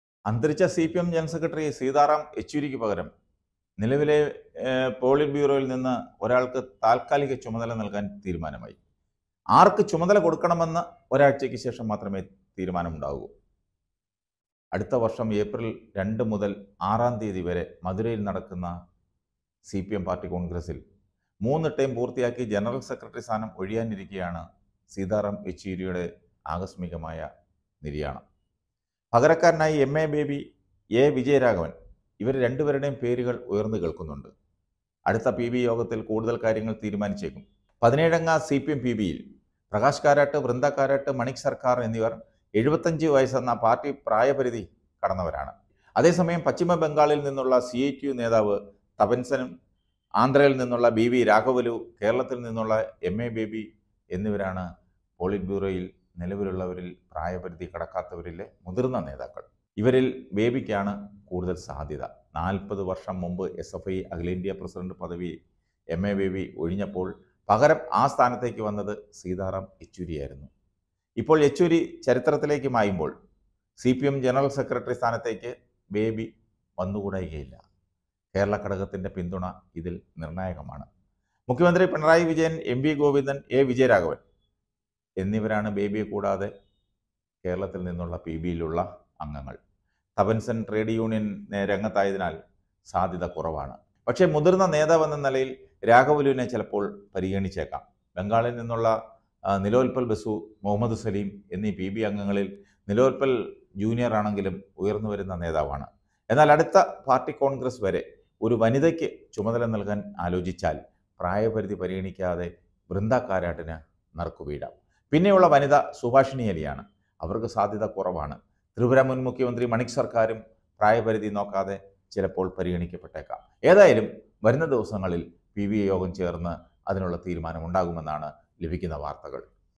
CPM | ജനറൽ സെക്രട്ടറിയായി എം എ ബേബി വരും , പി ബി യിലെ മുതിർന്ന അംഗം|LDF|CPI|CPIM
സിപിഎം ജനറൽ സെക്രട്ടറിയായി എം എ ബേബി വരും , പി ബി യിലെ മുതിർന്ന അംഗം